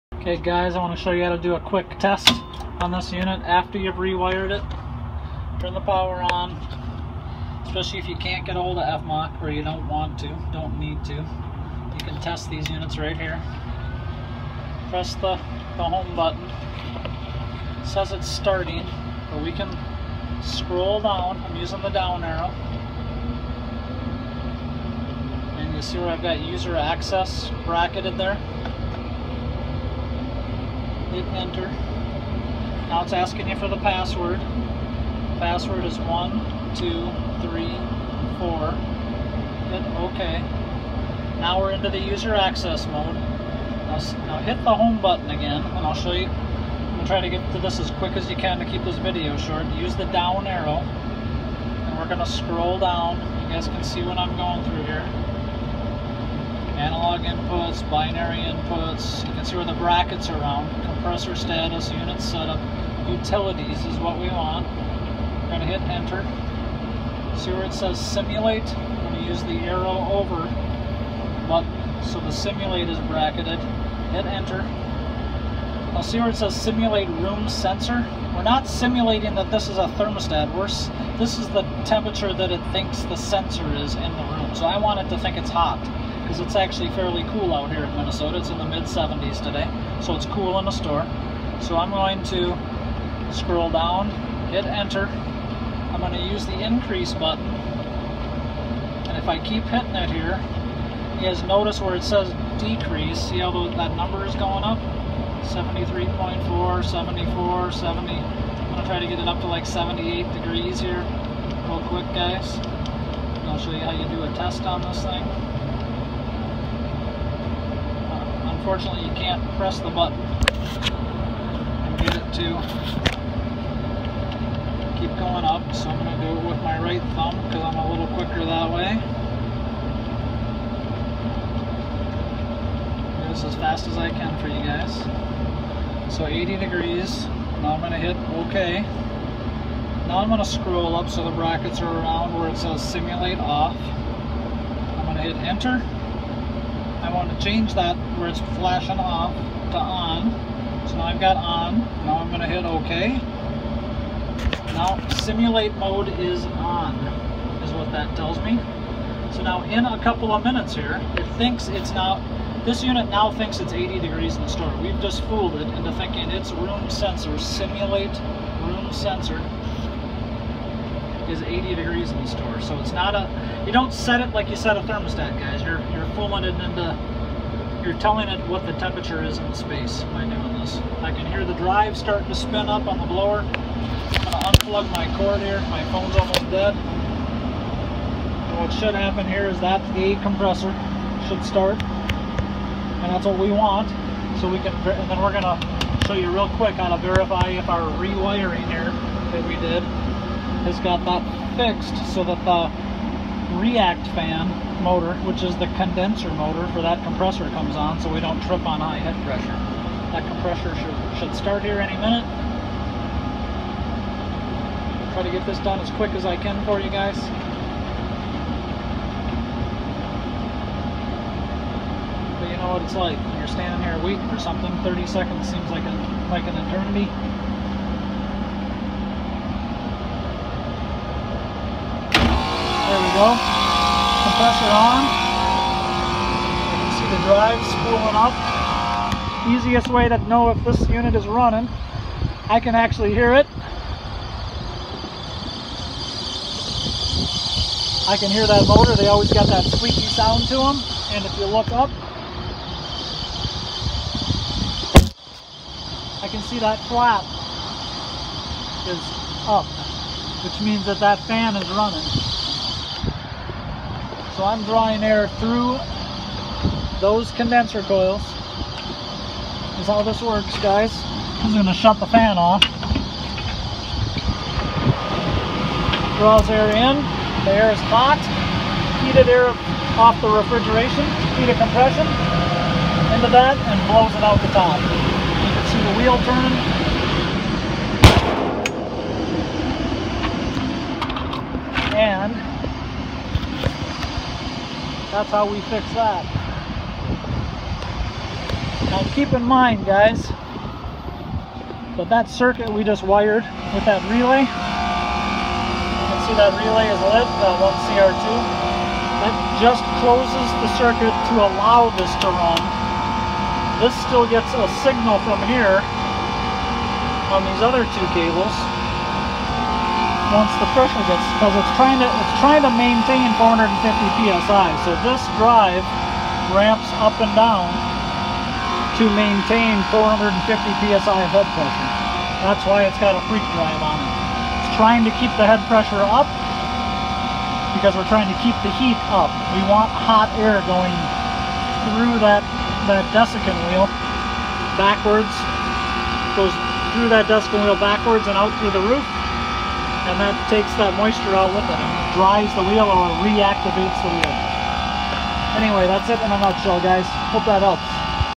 Testing Munters DHU / Using Simulate Mode
How to test Munters DHU after fixing flaws from a Door Rewire Project